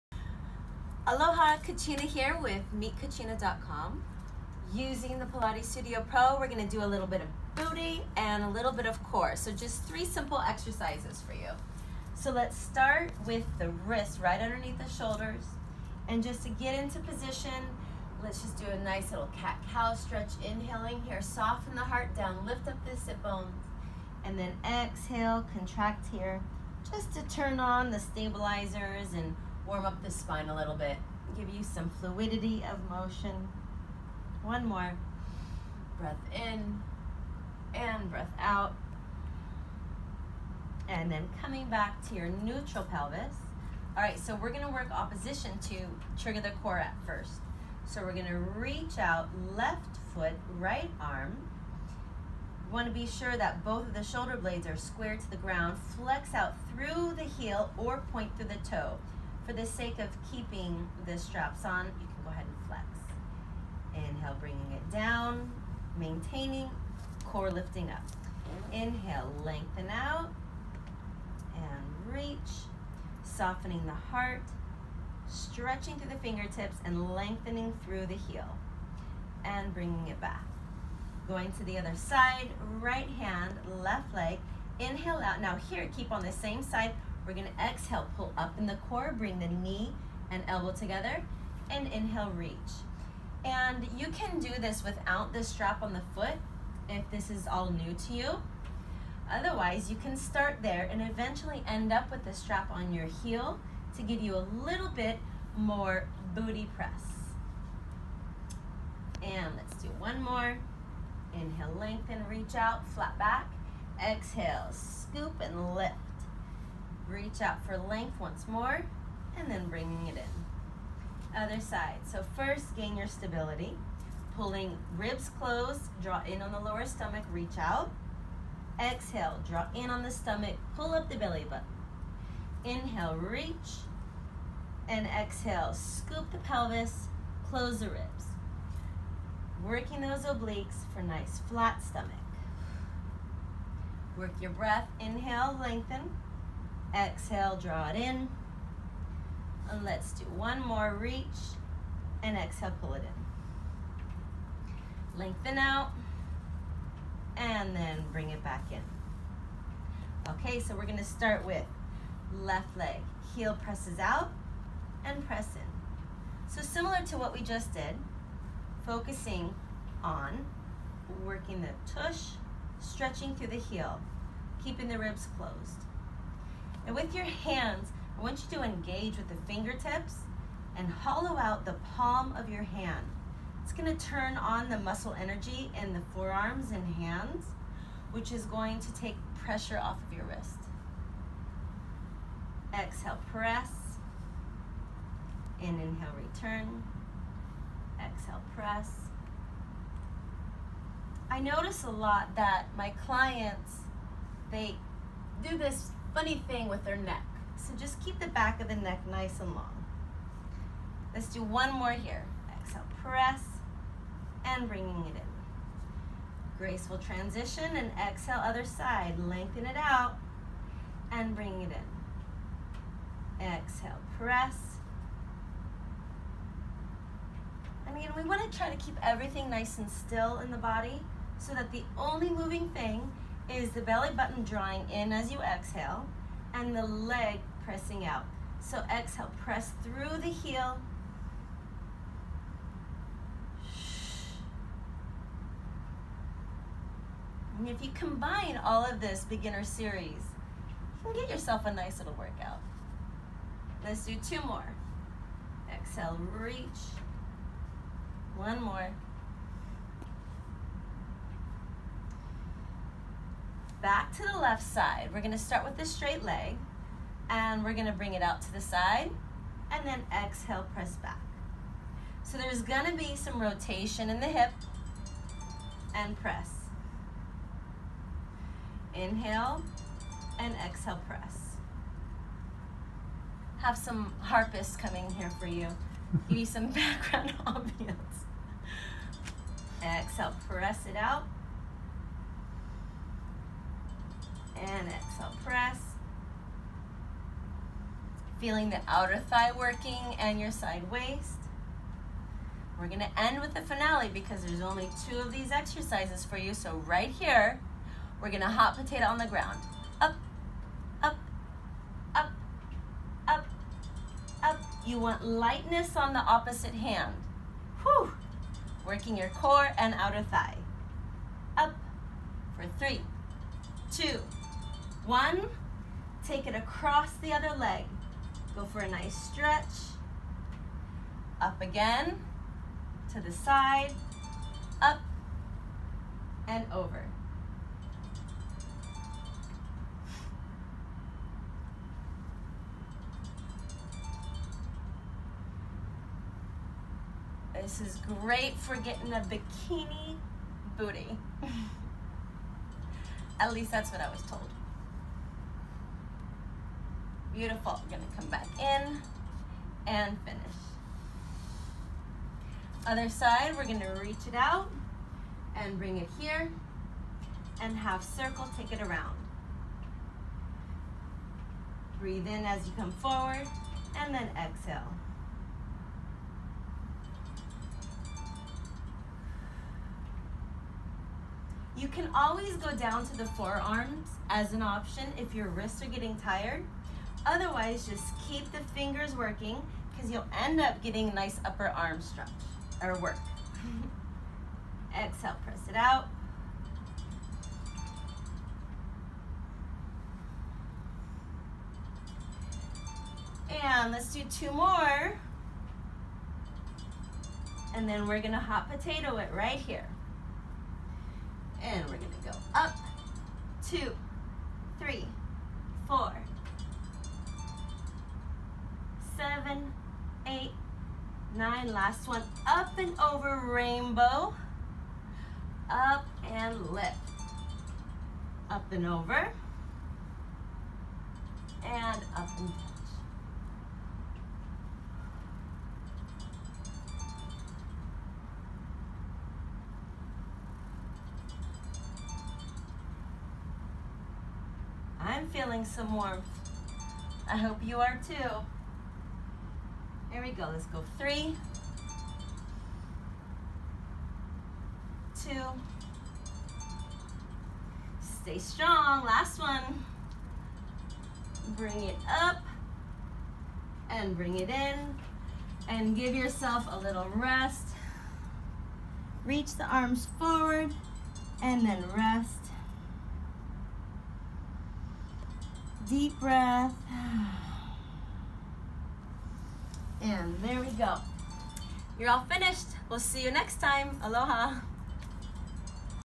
Beginner Instructional Pilates Booty and Core Burner Workout | PSPro™
It's always time to get your bikini body in shape.  Guys, no butt is not a good look.  This Pilates Studio Pro™️ workout will have your cheeks all loved, warm and rosy in 8 minutes!  Great glutes are for everyone!  Instruction by Movement Specialist Kachina Asher